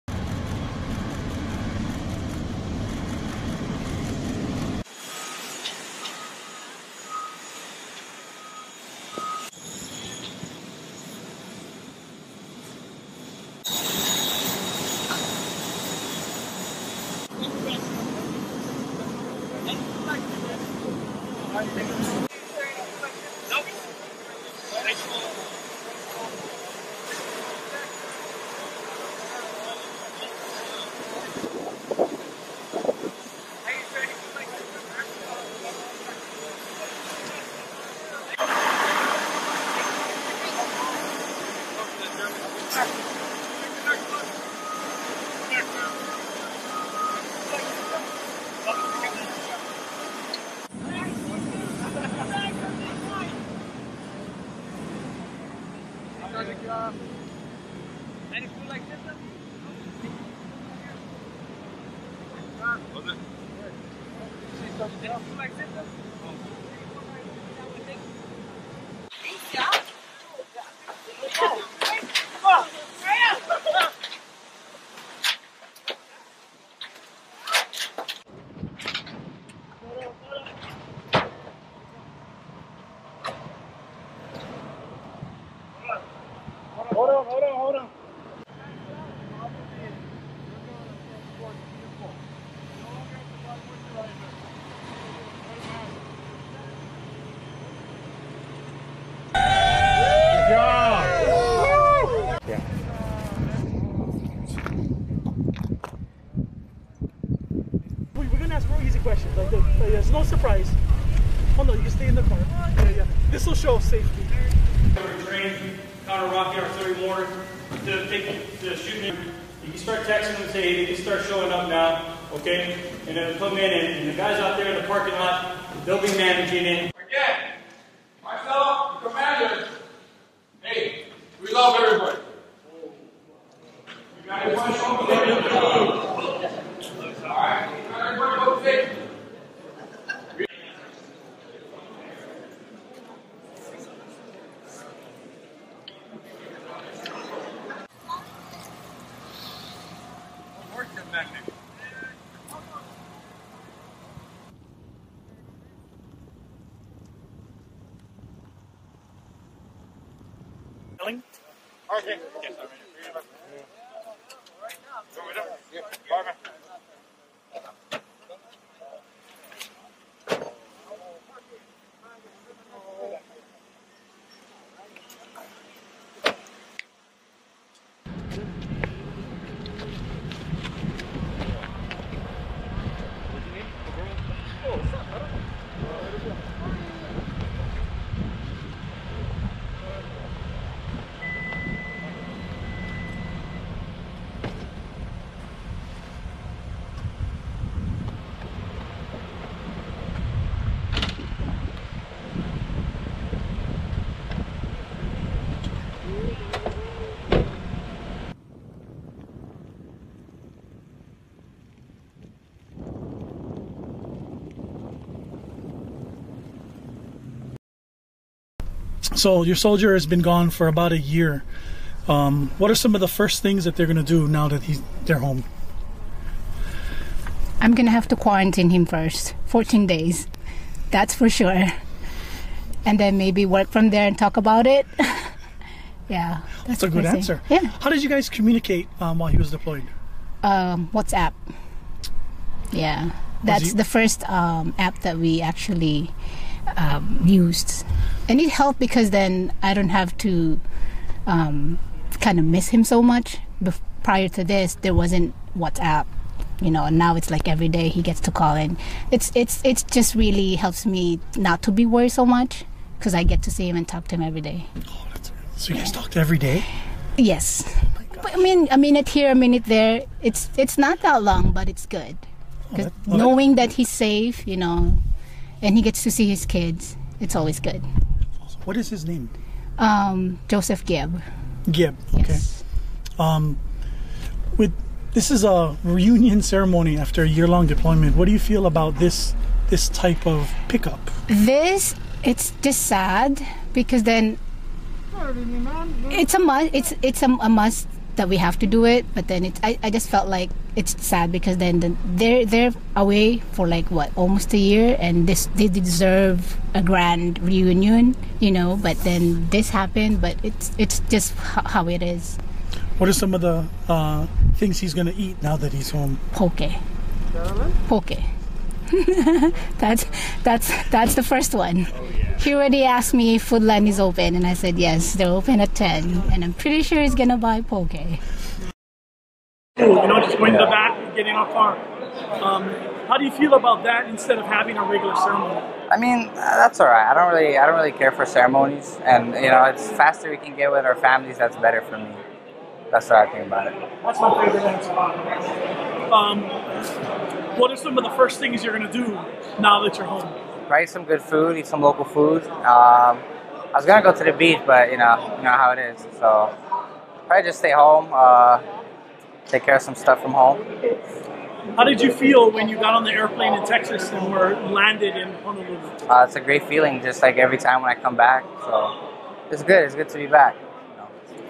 1-487 Field Artillery Return From Deployment
Credit: Tech. Sgt. Andrew Jackson | Date Taken: 04/15/2020
Hawaii National Guard Soldiers with the 1st Battalion, 487th Field Artillery Regiment return from their year long deployment to Afghanistan and Iraq, April 15, 2020, Honolulu, Hawaii. The Soldiers arrived in a commercial charter flight from Texas where they have been quarantined after their initial return from overseas. They were medically screened by fellow Hawaii Guardsmen who are conducting that support mission at the Daniel K. Inouye Honolulu International Airport in response to the COVID-19 pandemic. The Soldiers’ families waited in their cars outside the 29th Infantry Brigade Combat Team Readiness Center to pick up their returning Guardsmen. The Guardsmen were released six at a time and picked up with no one exiting the car. No hugs or greetings were allowed to insure physical distancing with their friends and family. It was an awkward redeployment event that resembled a drive-thru with the products being the happiest Soldiers in the world.  Package includes interviews with  00:03:37:14 Merlyn Guieb - Spouse of returning Hawaii National Guard Soldier  00:05:28:19 Spc. Jan (pronounced Yawn) Kryszkiewicz- returning 487 Soldier (U.S. Air National Guard video by Tech. Sgt. Andrew Jackson)